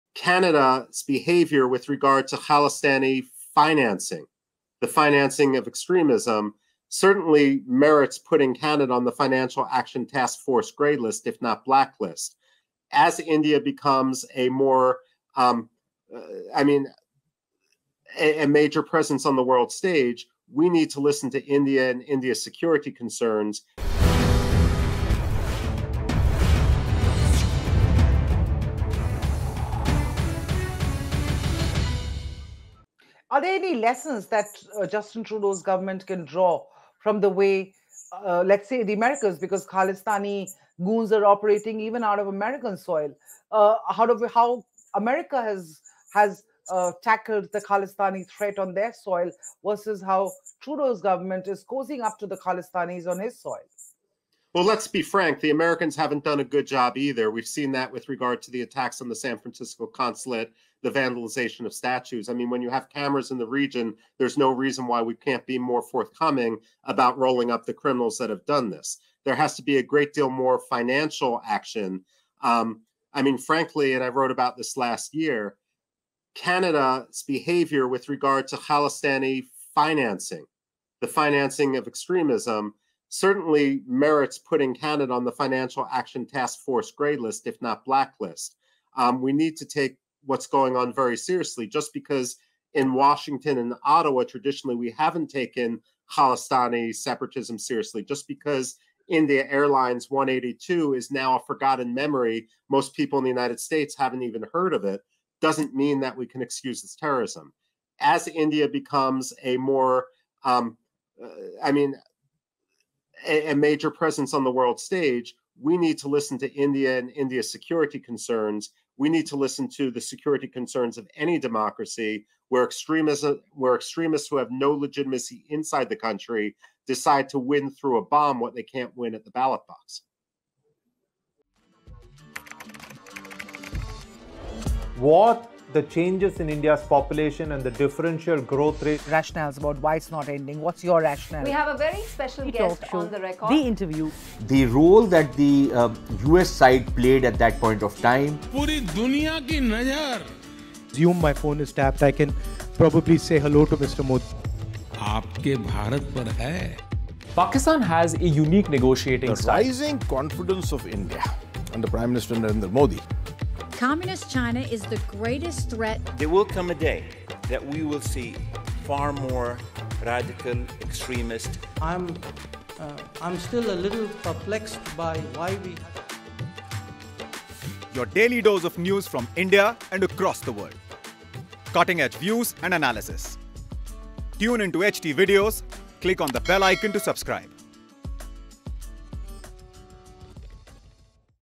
'Canada Financing Khalistan Extremism...': Ex-Pentagon Official Seeks FATF 'Grey List' For Ottawa
Amid diplomatic furore over Canada PM Justin Trudeau's unsubstantiated claim that Indian government agents had a hand in the killing of alleged Khalistani operative Hardeep Singh Nijjar on Canadian soil, former Pentagon official Michael Rubin says that Canada should be added to the Financial Action Task Force's 'grey list' for failing to check financing of Khalistani groups. One of the countries part of the infamous list, until recently, was Pakistan.